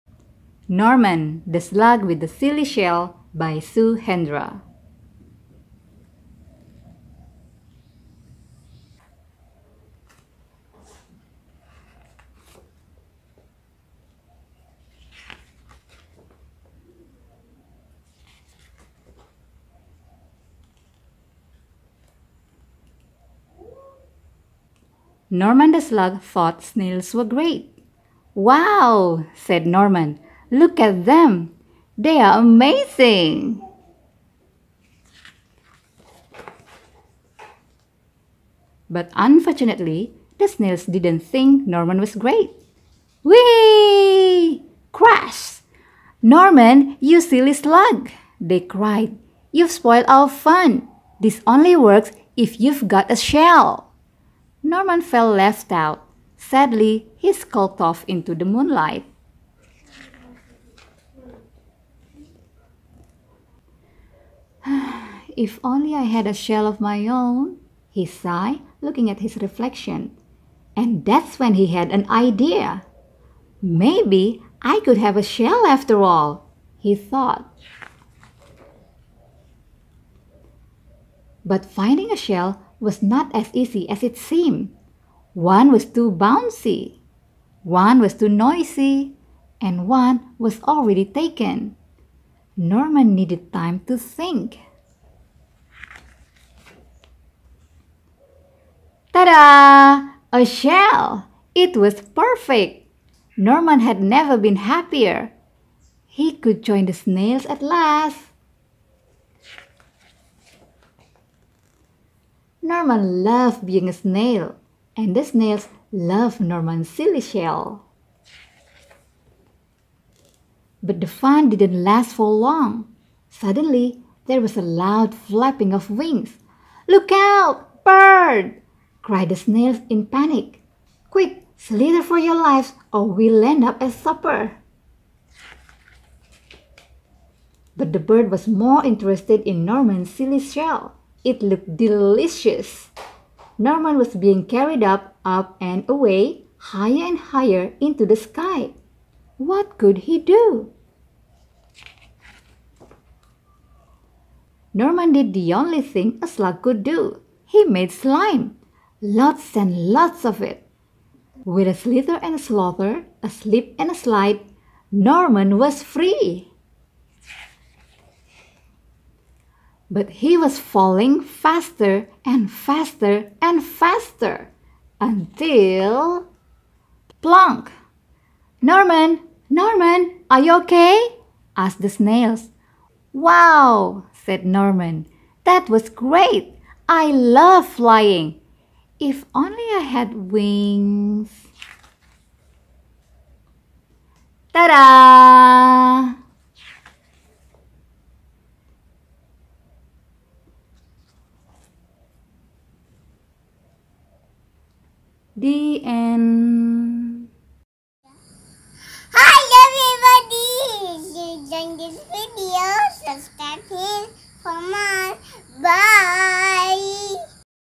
NORMAN - The Slug With The Silly Shell - Fun To Read Books
Meet NORMAN, the slug with the silly shell :)
Norman is a slug, which has no shell, for sure. How can Norman has a shell? And why does he want to have a shell? How silly his shell could be?

Let's read aloud together to find out.
This story is one of fun to read books written by Sue Hendra.
If you like funny animal stories or funny bedtime stories, then this story book is for you.